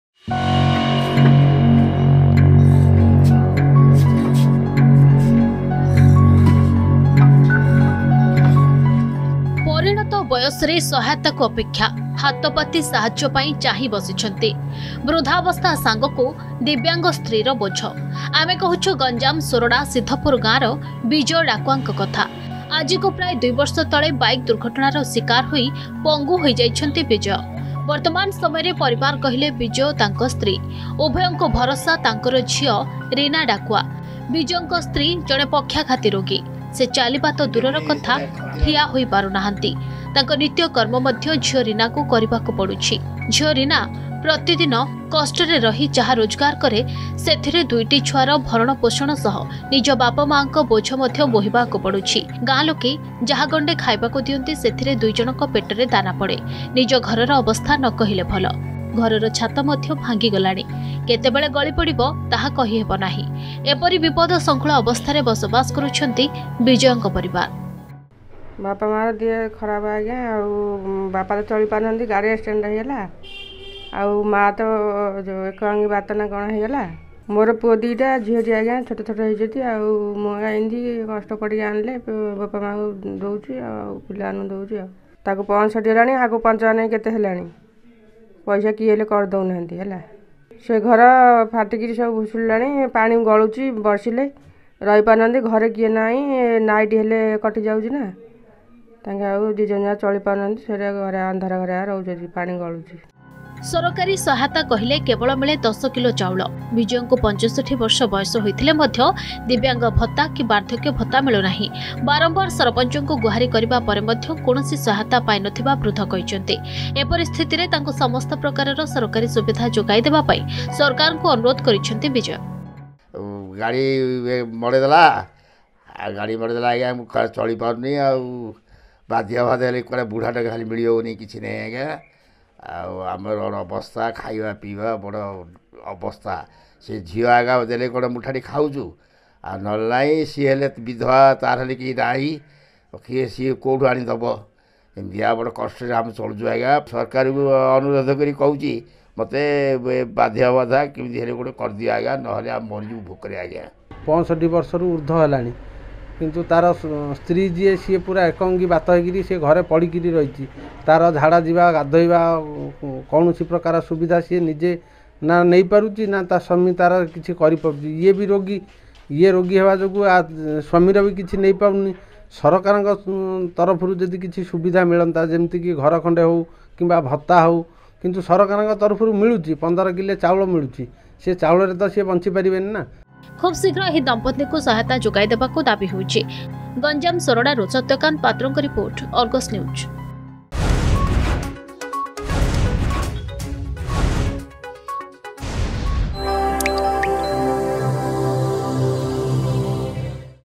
ପରିଣତ ବୟସରେ ସହାୟତାକୁ ଅପେକ୍ଷା
ପରିବାର ଭରଣ ପୋଷଣ ପାଇଁ ସବୁ ବେଳେ ସଂଘର୍ଷ କରିବାକୁ ପଡ଼ିଥାଏ କିନ୍ତୁ ଯେତେବଳେ ପରିସ୍ଥିତି ମନୁଷ୍ୟ କୁ ପଙ୍ଗୁ କରିଦିଏ ସେତେବେଳେ ସହାୟତା ପାଇଁ ଅପେକ୍ଷା କରିବାକୁ ପଡେ। ଏପରି ଏକ ଅଭାବିନୟ ଘଟଣା ଦେଖିବାକୁ ମିଳିଛି ଗଞ୍ଜାମ ଜିଲ୍ଲା ସୋରଡା ର ସିଦ୍ଧପୁର ଗ୍ରାମରେ। ଆଜକୁ ପ୍ରାୟ ଦୁଇ ବର୍ଷ ହେବ ଗୋଟିଏ ପରିବାରର ଜଣେ ବୃଦ୍ଧ ଓ ବୃଦ୍ଧା ସହାୟତା ପାଇଁ ଚାହିଁ ବସିଛନ୍ତି।